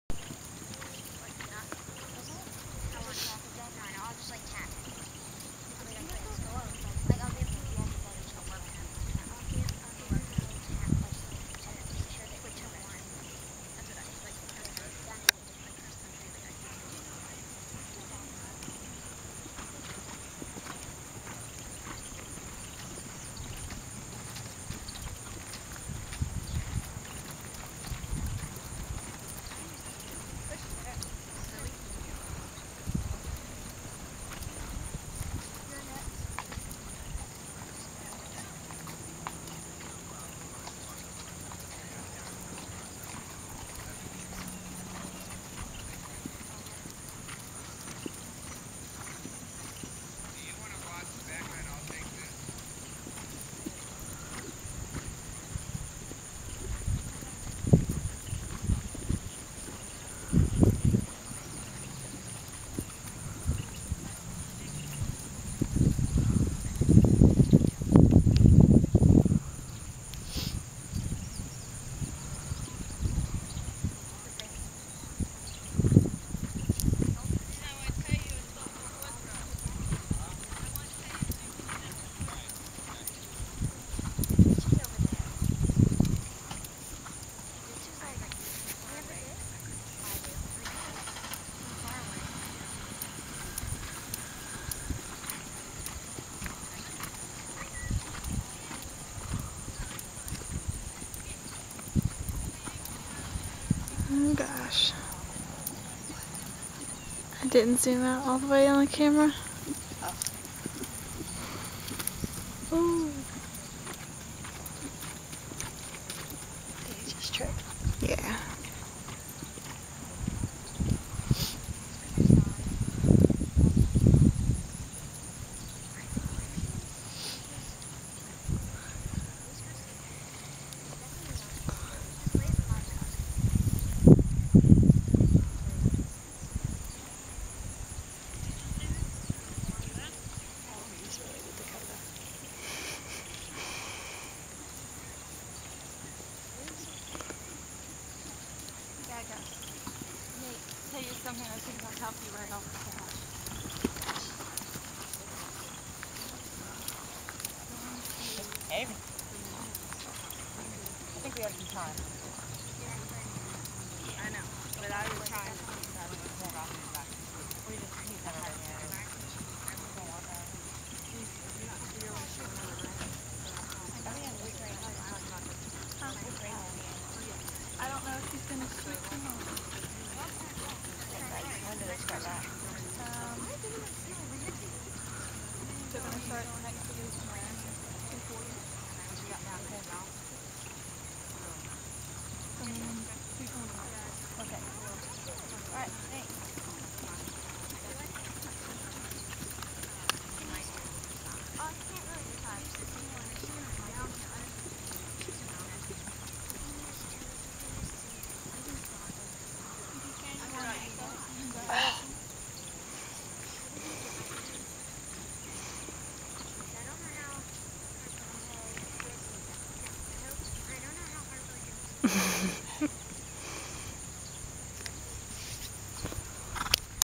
Moon D2
Megan and Moon dressage at River Glen